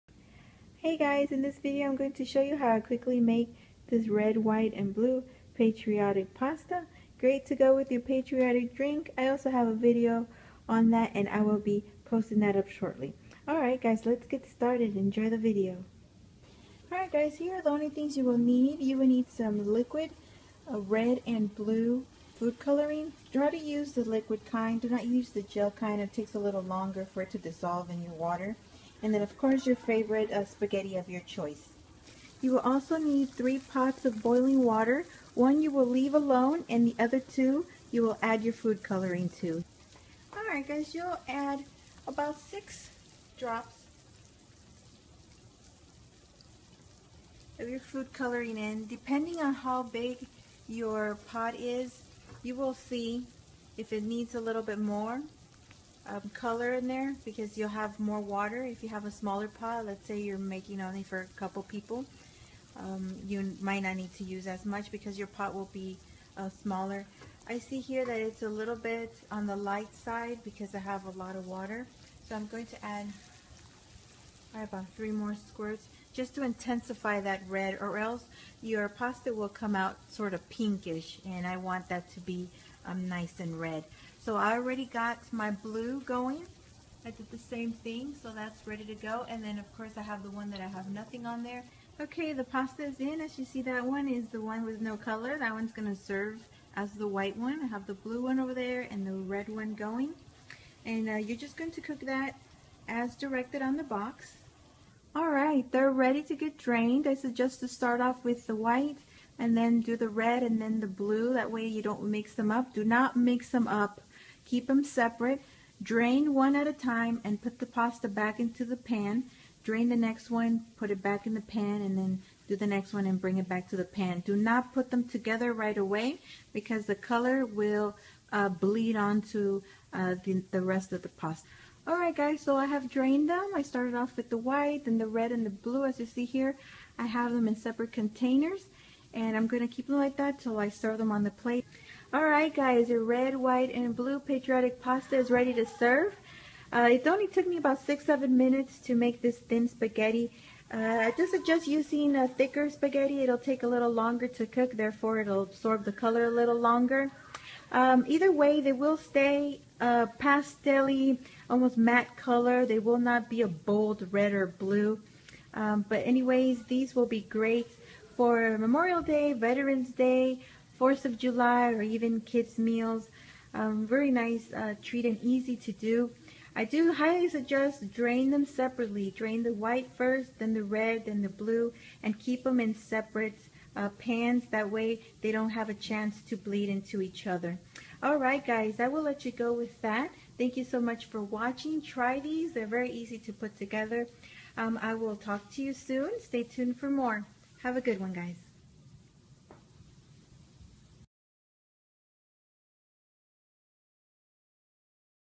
How To Make Patriotic Red White & Blue Spaghetti Pasta
Hey everyone, here is a video on how I make colored pasta..I'm using red, white, & blue for Memorial Day, but you can easily make any other color..Hope you like the video & give this a try, kids love colorful foods & this is an easy way to make some...Thanks again for watching :)